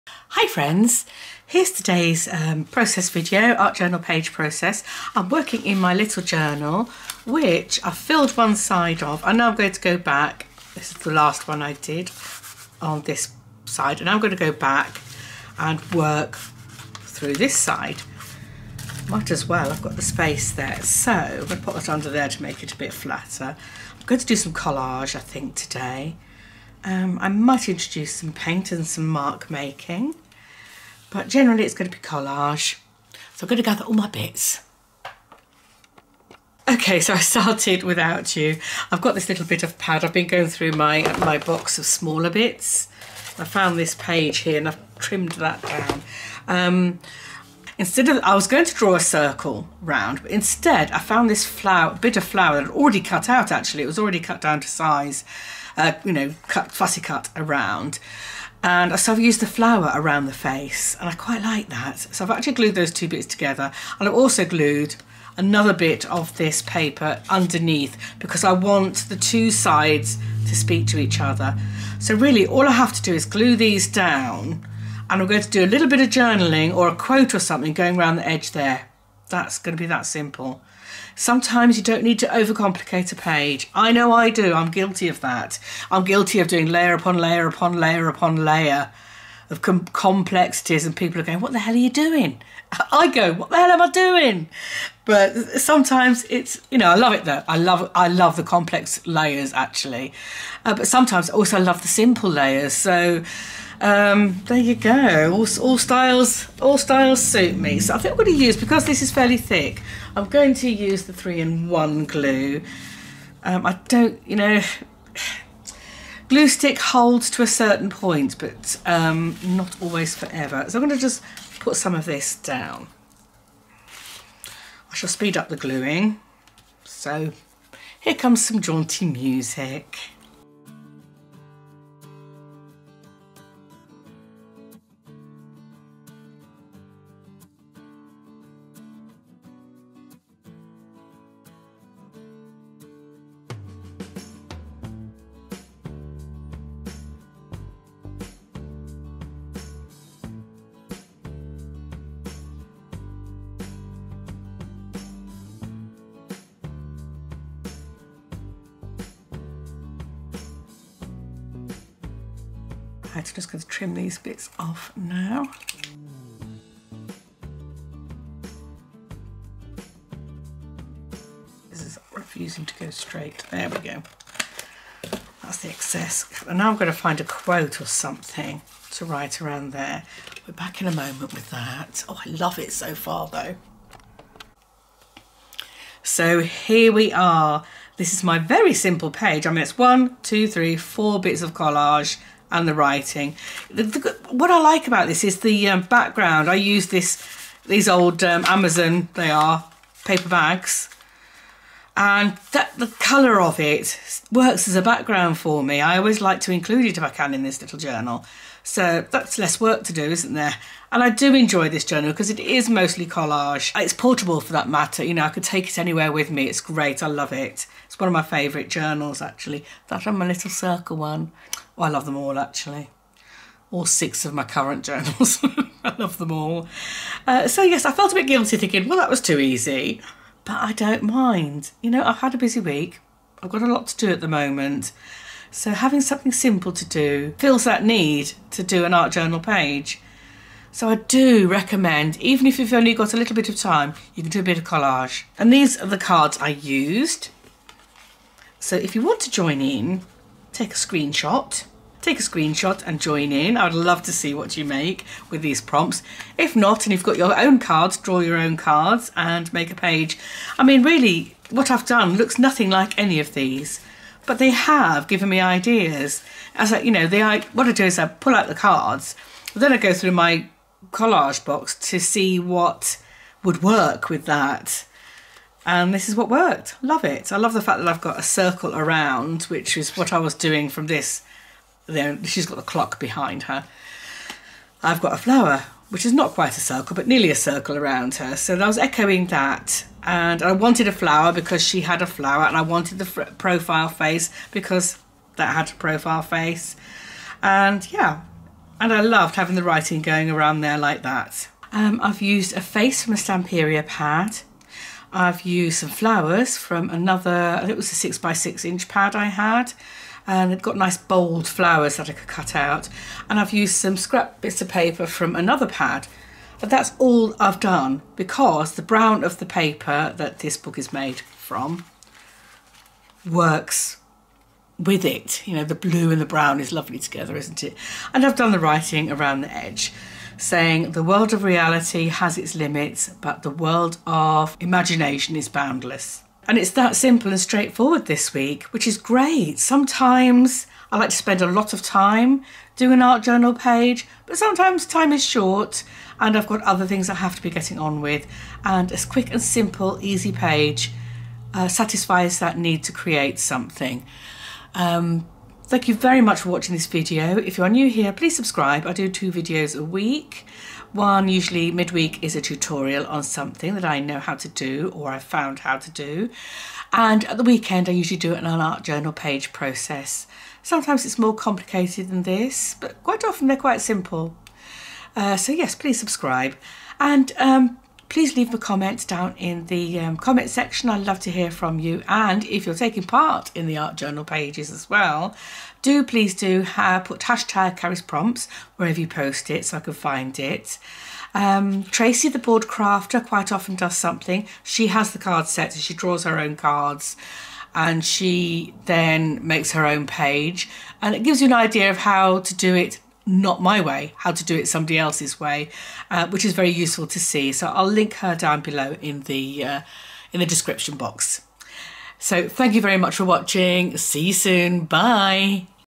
A Quick Collage Art Journal Page
Sometimes we only have a short window of time to make an art journal page and this is where I find collage very helpful. This is a very quick page using just a few collage elements - I love it!

Tracey from The Bored Crafter creates pages from the prompts cards too and you can see her style

💗My Etsy Shop for my own unique stencil designs, downloadable prints and the occasional sticker: